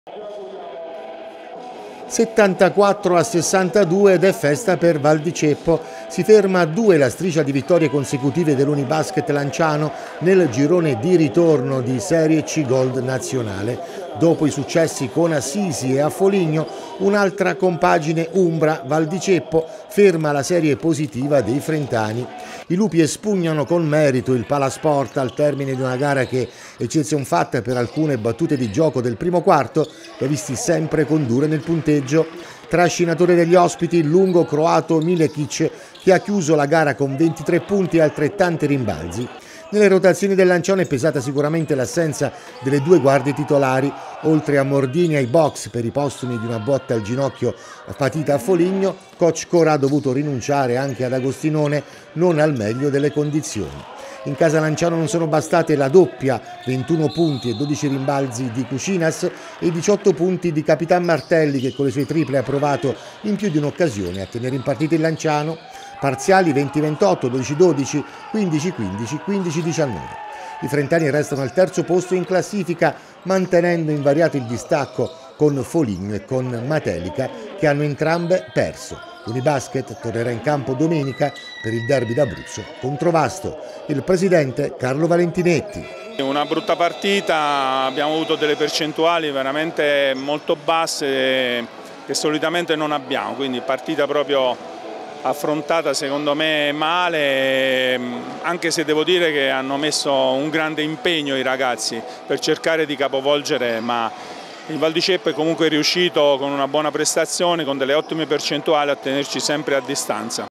Unibasket Lanciano - Valdiceppo 62 - 74
Basket: in serie b vincono Chieti e Giulianova, Teramo affossato. In C Gold Unibasket Lanciano battuta in casa dal Valdiceppo.